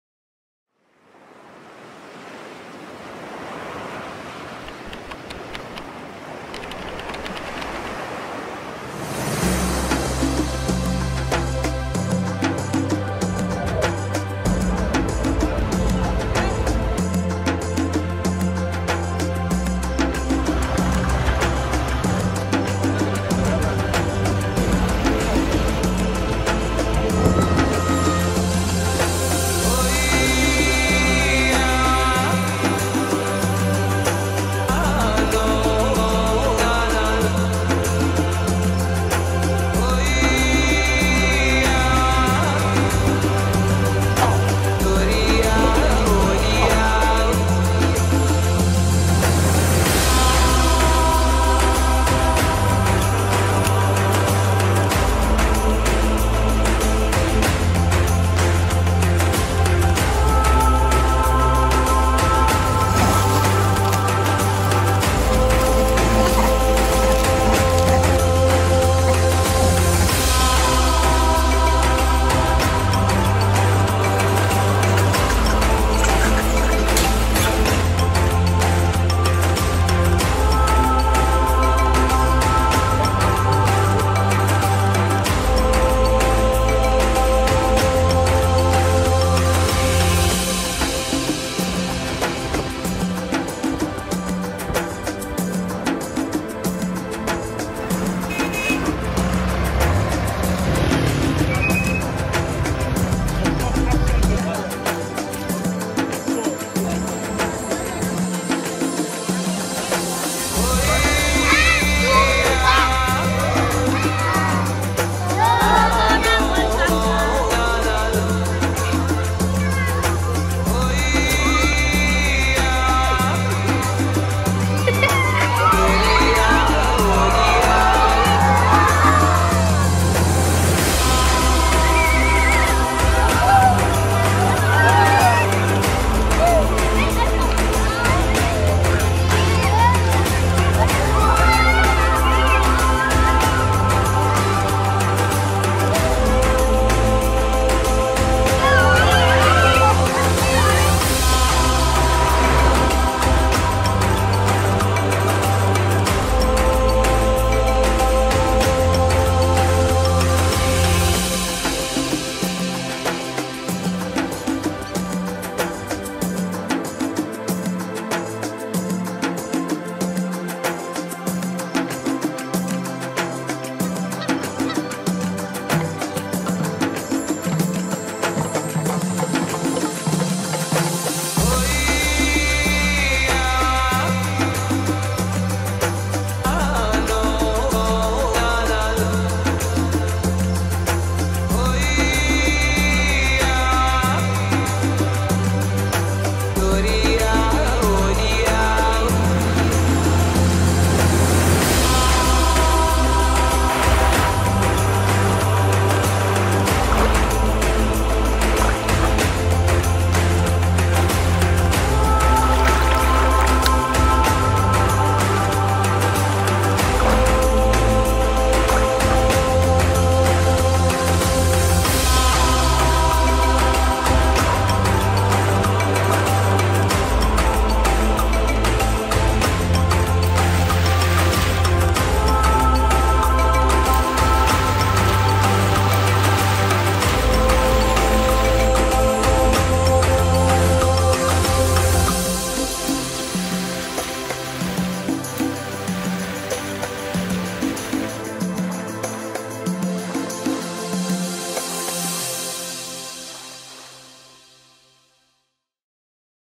THE GAMBIA by DRONE in 4K
Locations:
Kololi
Banjul
Barra
Fathalla (Senegal)
Bijilo National Park
Farafenni
Porli School
Bakadaji Kunda
Lamin Koto
Stone Circles, Wassu
River Gambia National Park

Cameras:
DJI Mavic Air 2
DJI Avata
iPhone 12